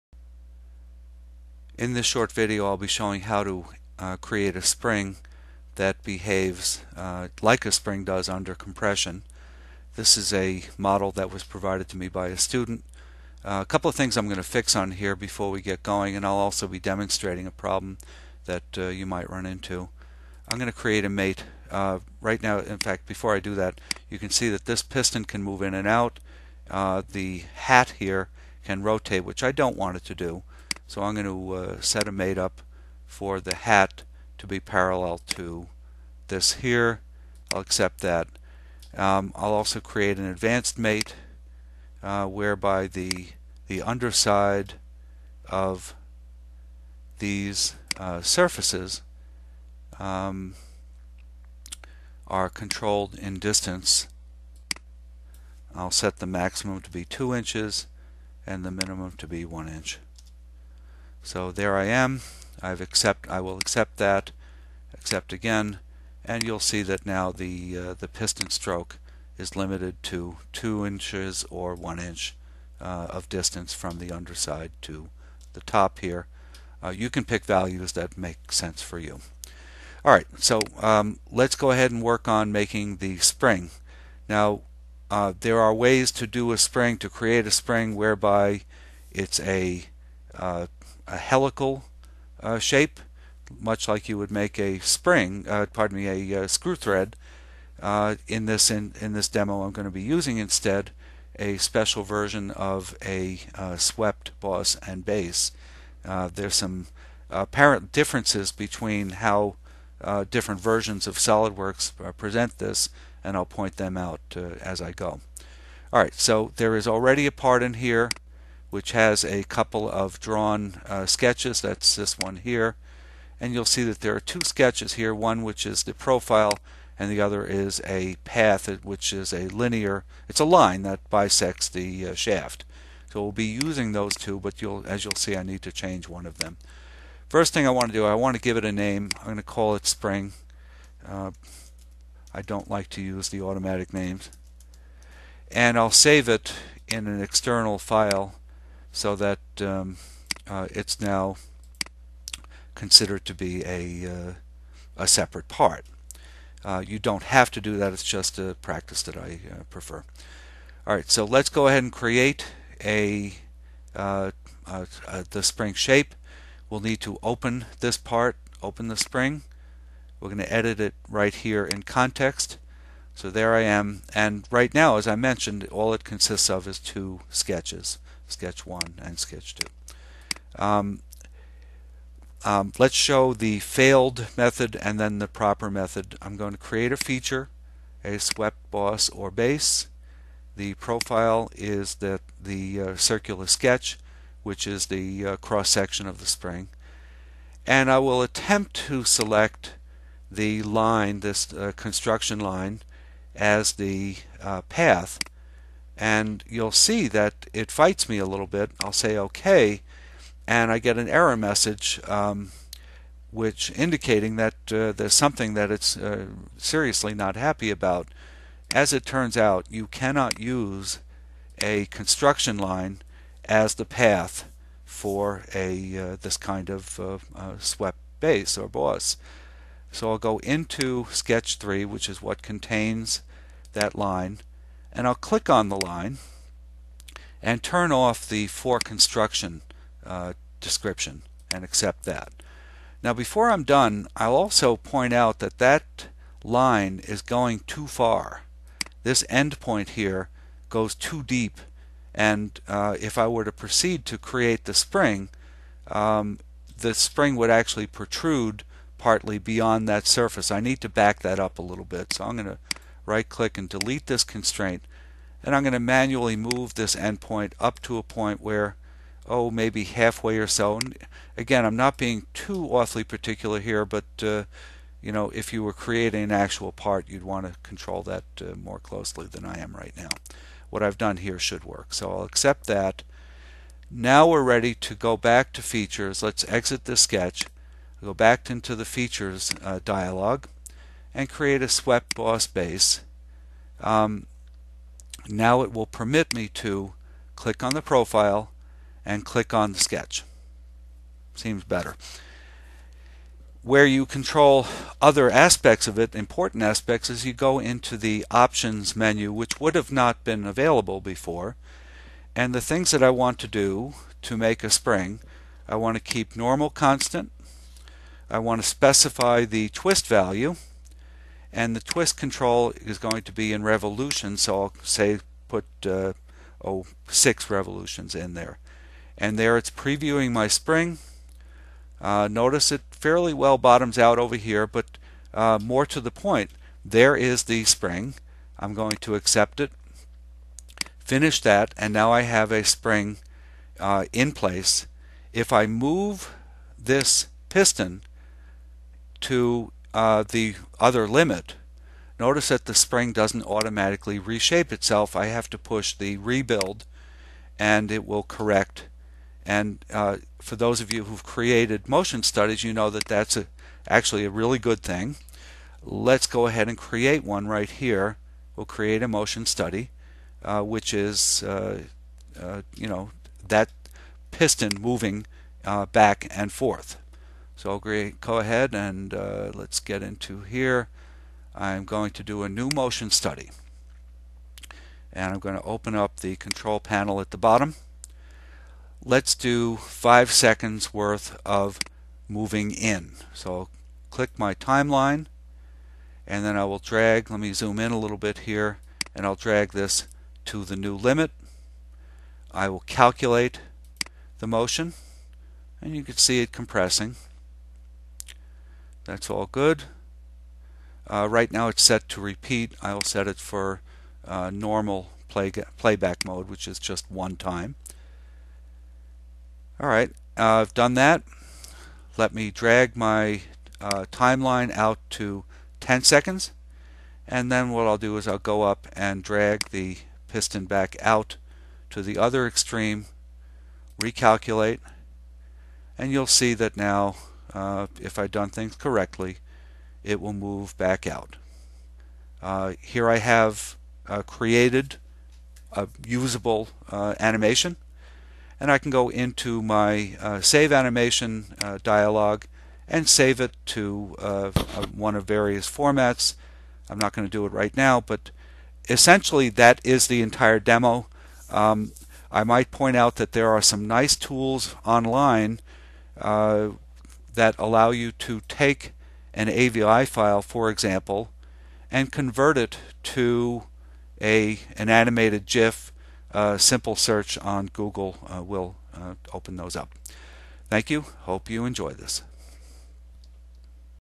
Solidworks: Create Spring and Motion Study
A demo for my students, illustrates using one method of Swept Boss/Base to create a spring that lends itself well to animation.